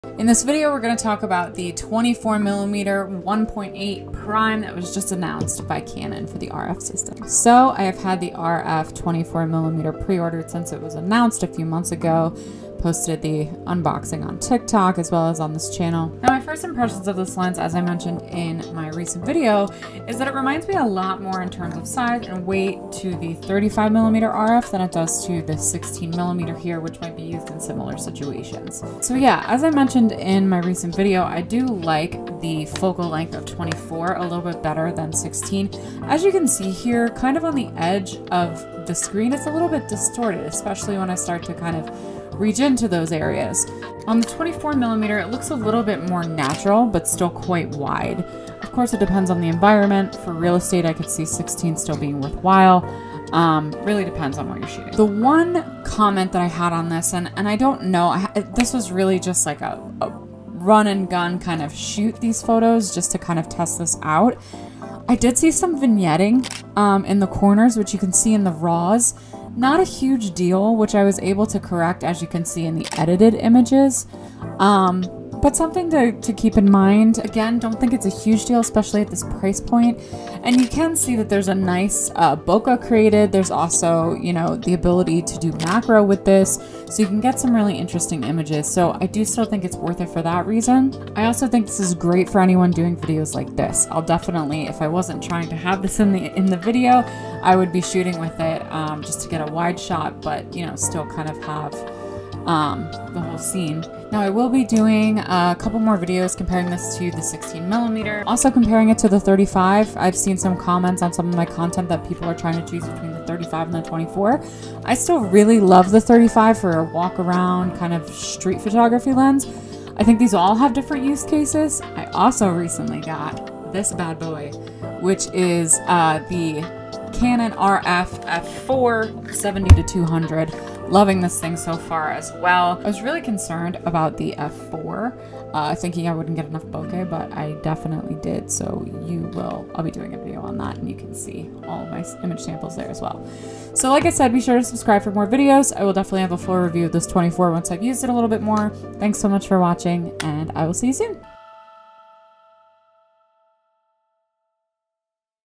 Testing Canon's NEW RF 24mm f/1.8! Sample Photos & First Impressions
Sharing my first impressions of the Canon RF24mm F1.8 Macro IS STM lens and showing you some test photos.